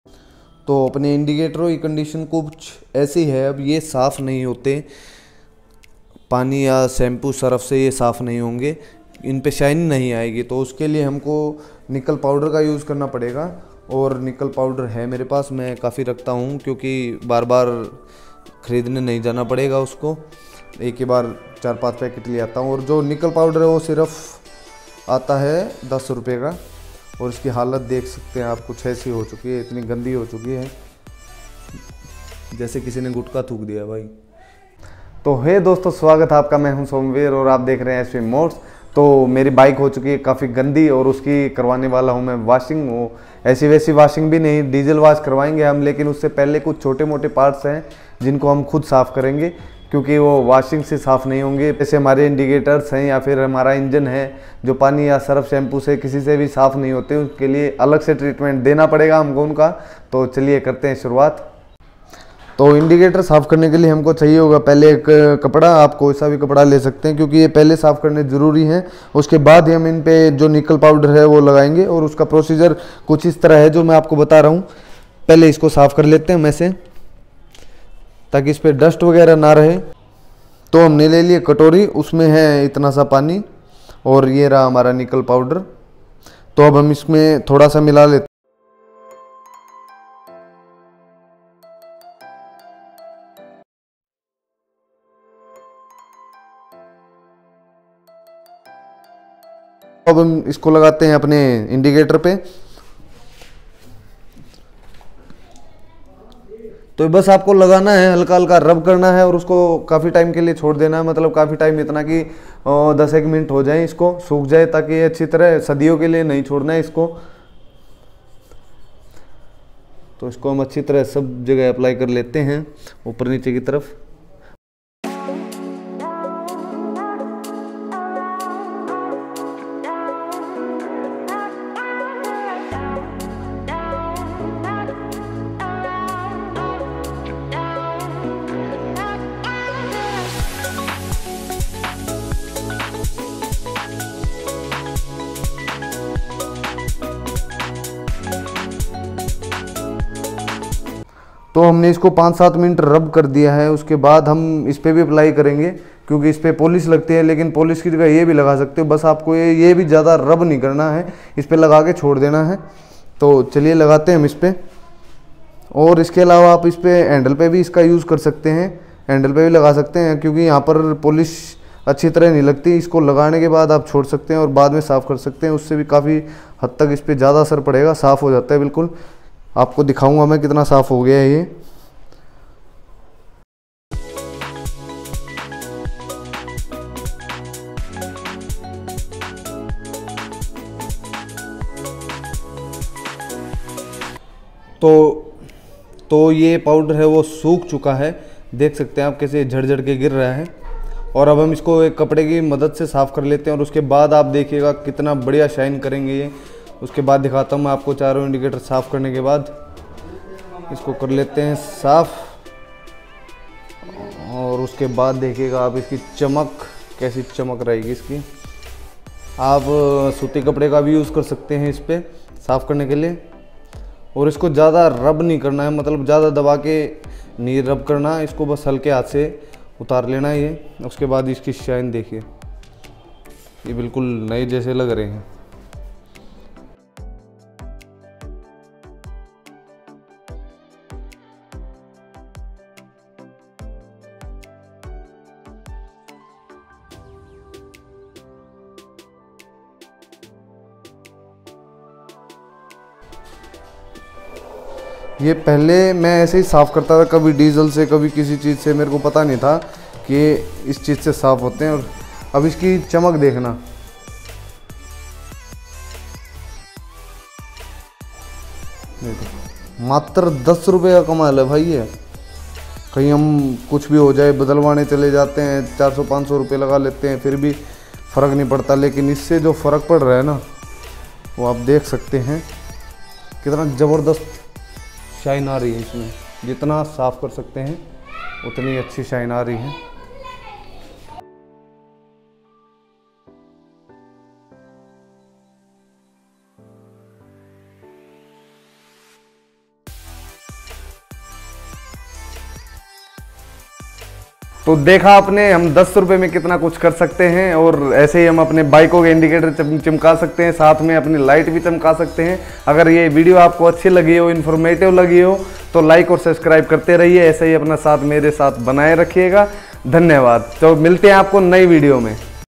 How to maintain bullet chrome parts from rust in 10 ₹ only
How to maintain chrome parts from rust @MASTANAMUSAFIR1
Nickle powder pouch -10₹only
First mix it with water then apply on chrome surface and rubb it for 5 6 min from light hand
Wait for 10 min
Remove from cotton clothe